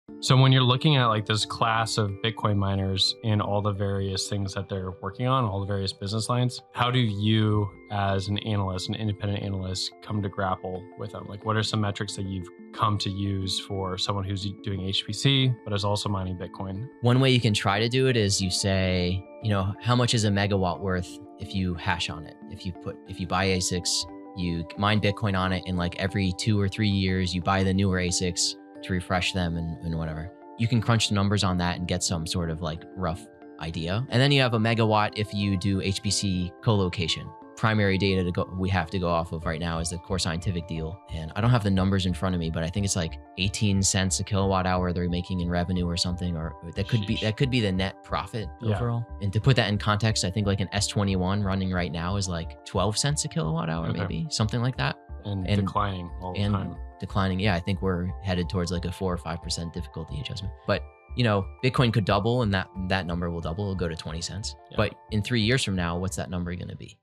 The Mining Pod
Whether it’s mining bitcoin or selling HPC/AI computing power, public bitcoin miners are in the business of monetizing megawatts.
“How much is a MW worth if you hash on it” versus “HPC colocation”?

Watch Pennyether on the latest Bitcoin Stock Show break down valuing MW allocations for hashrate vs. AI/HPC